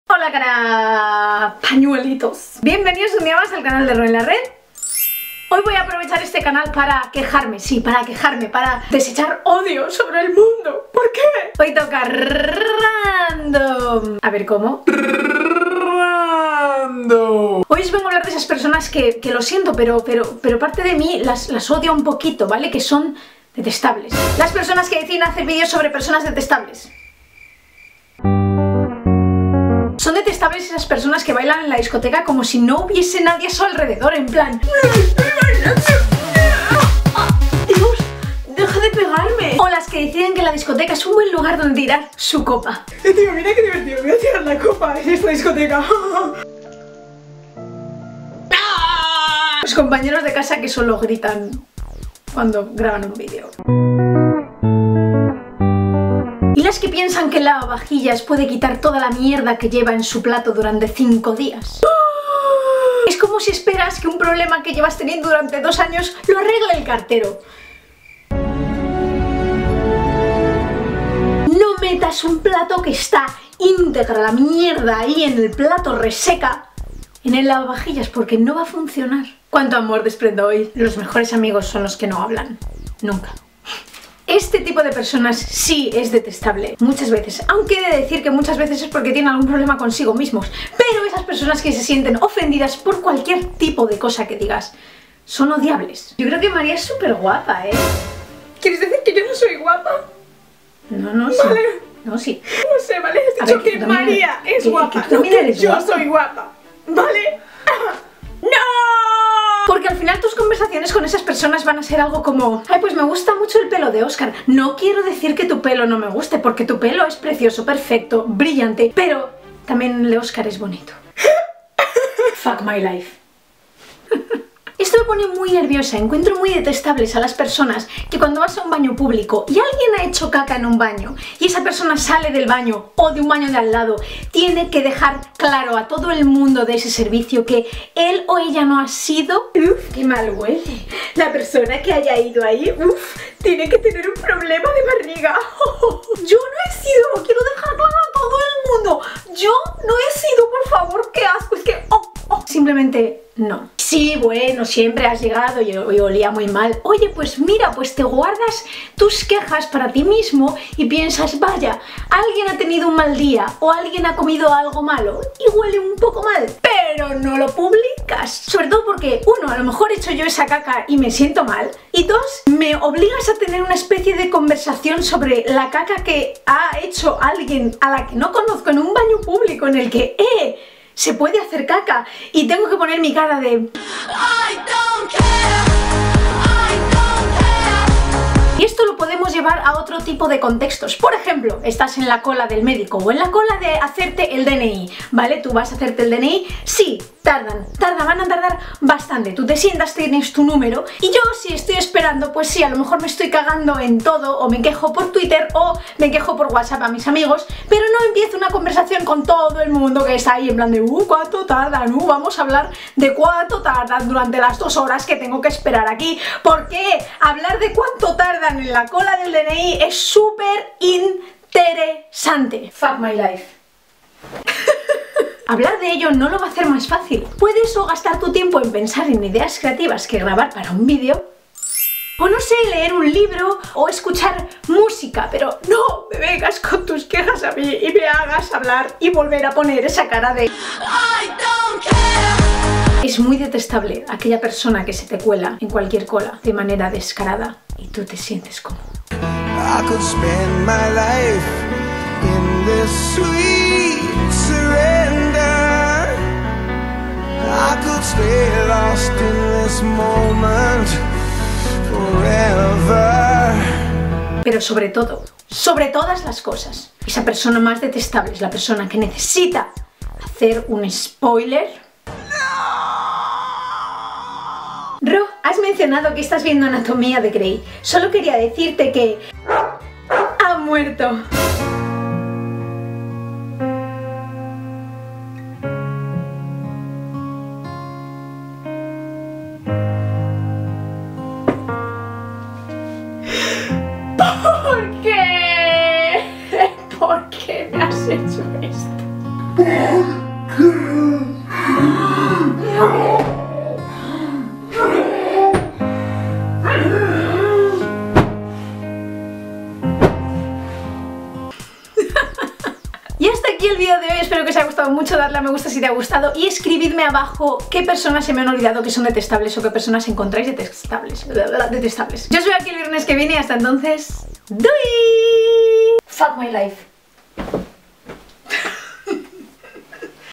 Personas que odio | Roenlared
Hoy quiero contaros cuáles son las personas que más detesto y odio. ¿Cuáles se me han olvidado?